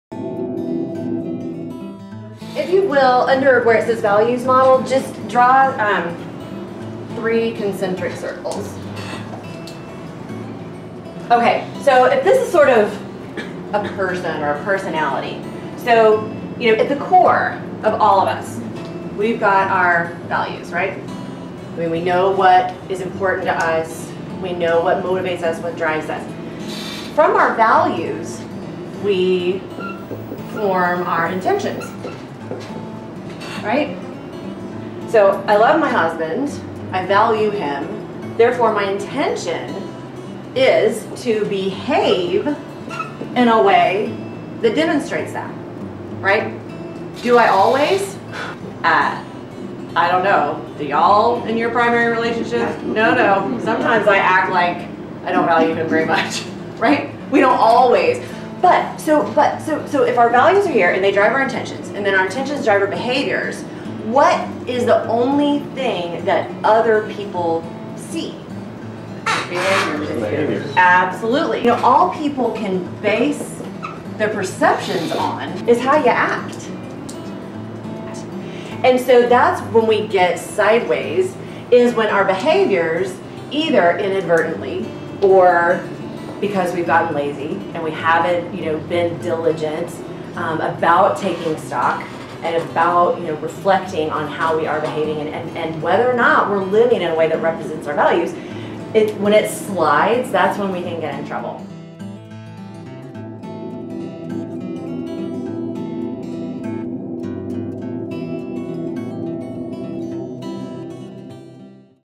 Values Model
Leadership Expert Julie Dunn (of Southwest Airlines) explains how we can build healthy relationships at work and manage conflicts that happen because of a gap in communication.